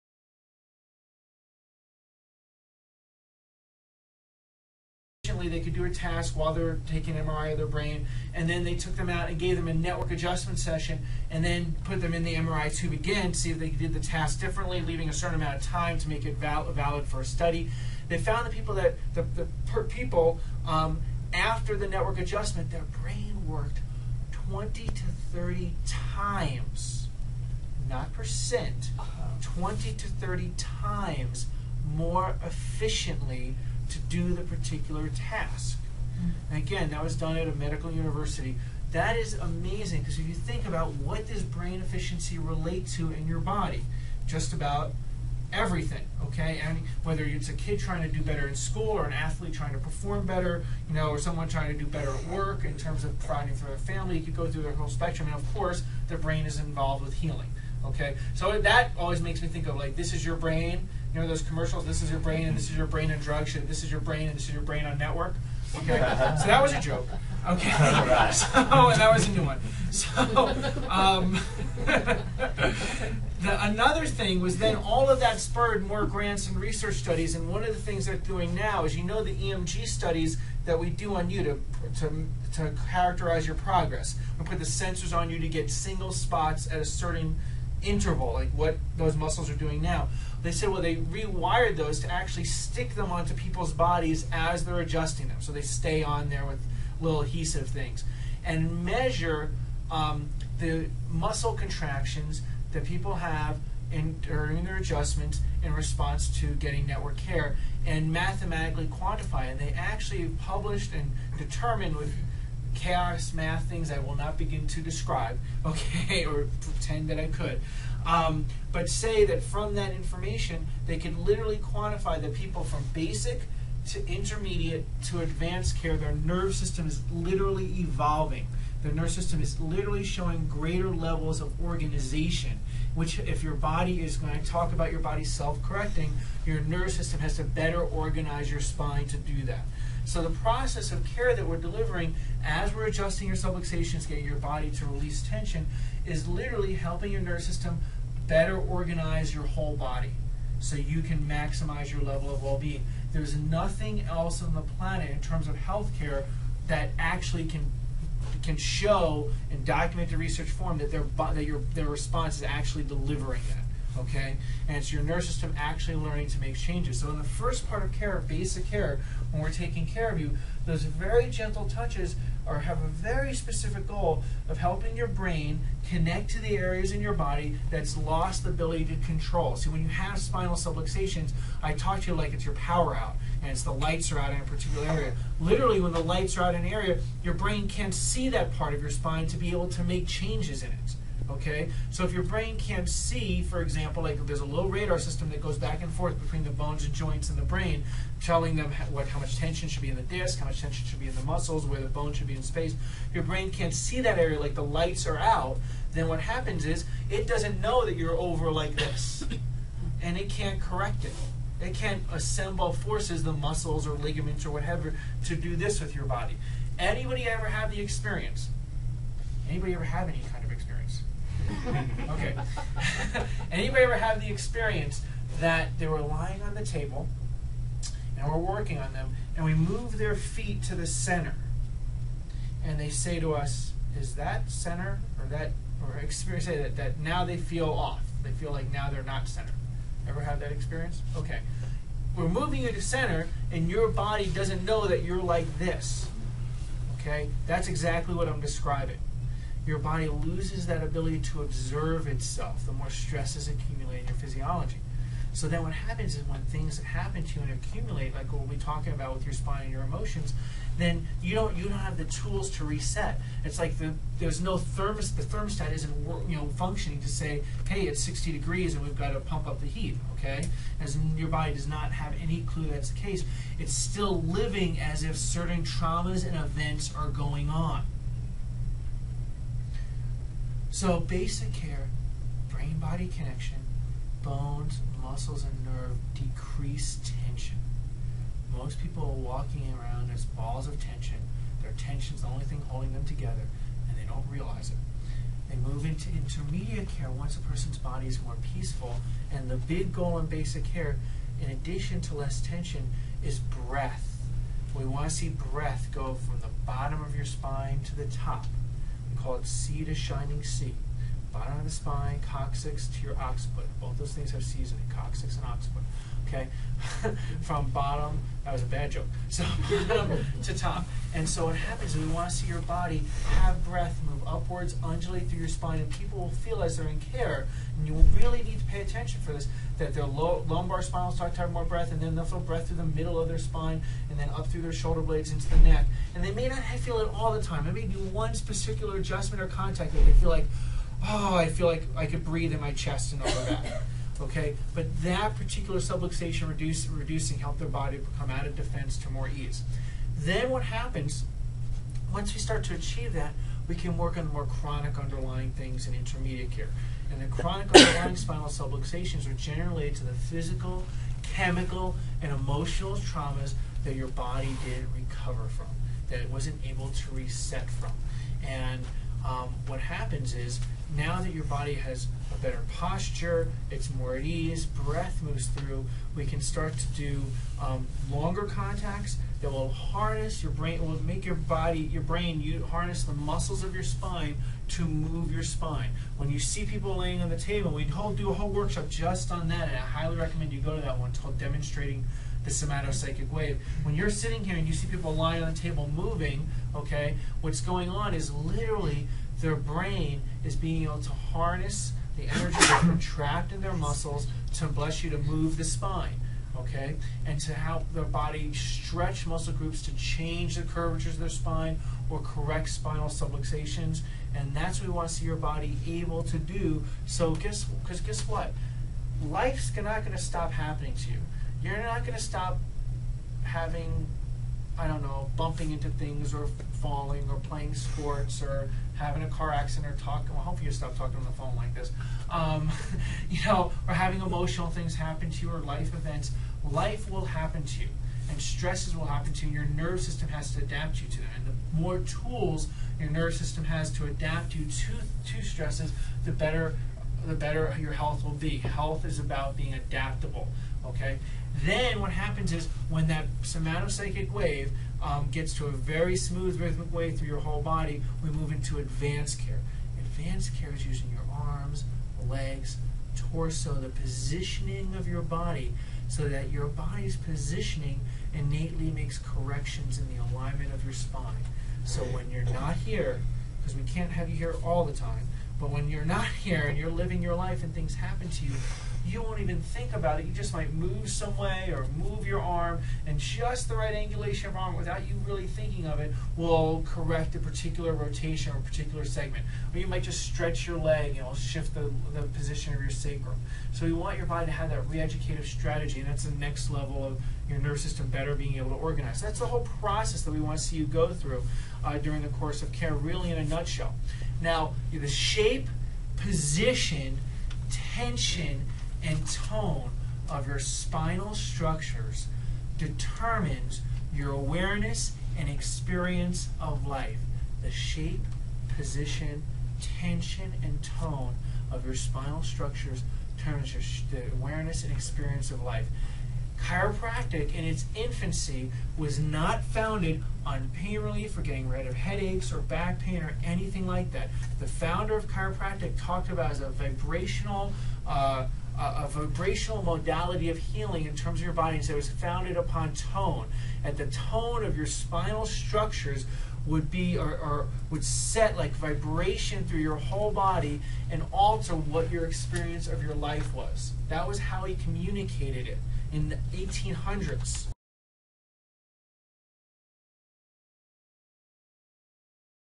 Boulder Chiropractor | Network Chiropractic | Spine & Emotions 2
Boulder Chiropractor| Spine and Emotions workshop clip 2 from  Boulder Chiropractors Drs. Daniel & Richelle Knowles at Network Family wellness Center.